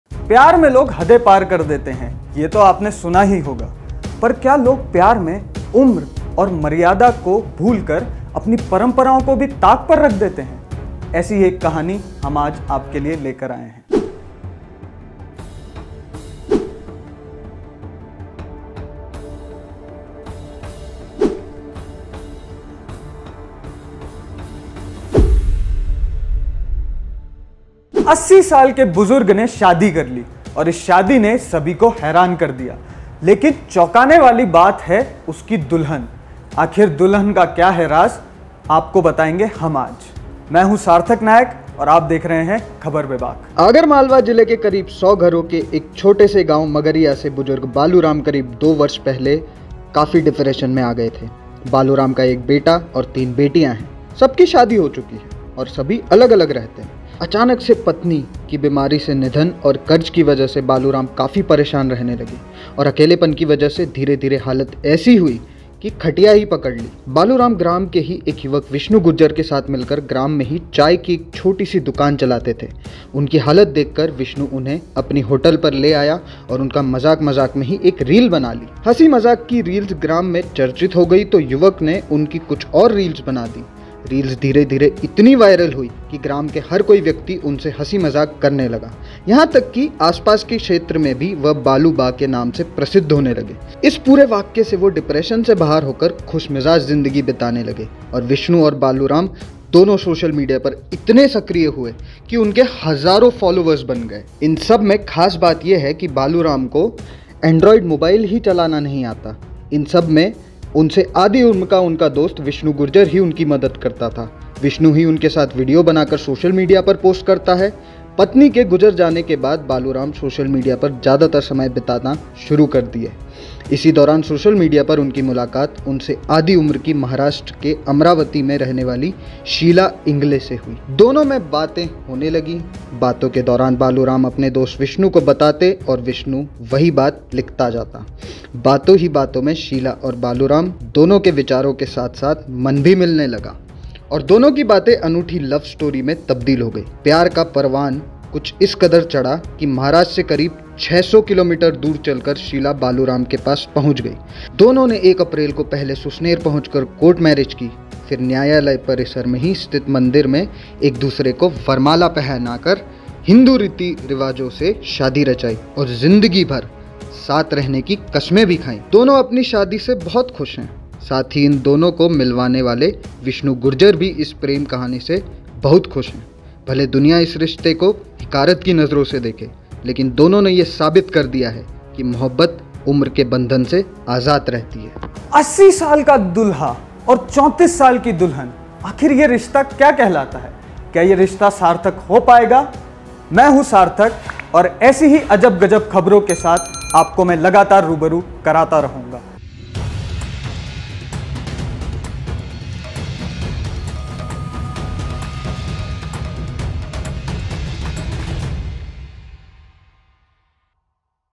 Old Man Marries Young Woman: 80 साल के बुजुर्ग के प्यार में डूबी जवान महिला, फिर कर दी सारी हदें पार
IBC24 के लोकसभा चुनाव Survey में हिस्सा लें और जीतें लाखों के ईनाम